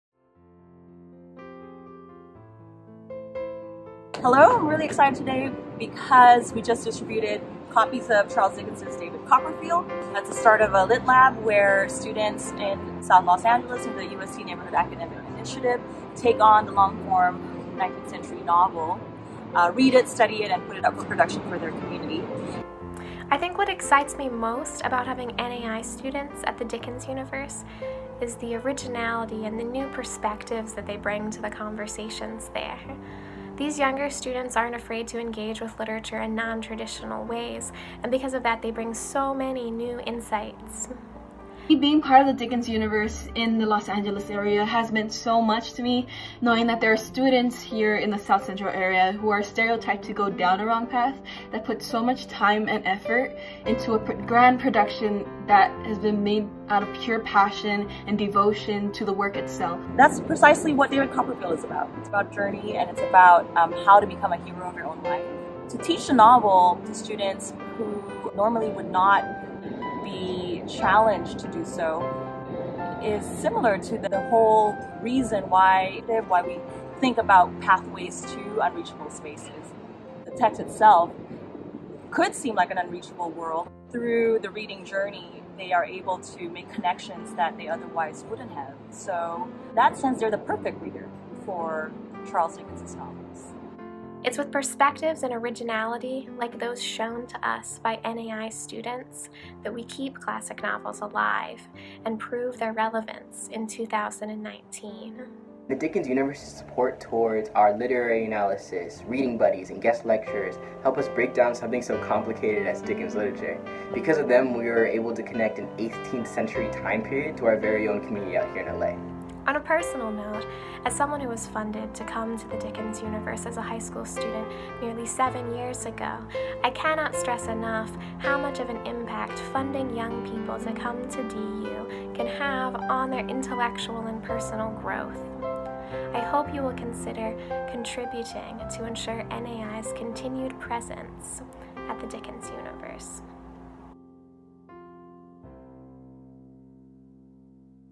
2019 Friends Giving Week - Dickens Project High School and Teacher Scholarship Program
Since 2015, the Dickens Project has partnered with the Neighborhood Academic Initiative (NAI) in Los Angeles to bring students and teachers for a week of intensive study at the Dickens Universe conference. For the first-generation students who attend the Dickens Universe, it's a unique opportunity to taste the college experience at UC Santa Cruz, alongside international faculty, graduate students, and Victorian literature aficionados.

Your gift will provide critical support to these students and teachers and will allow us to expand this unique program to more students at high schools in other communities.

Please show your support for the Dickens Project High School and Teacher Scholarship Program by making a donation during Friends Giving Week, October 24-28, 2019.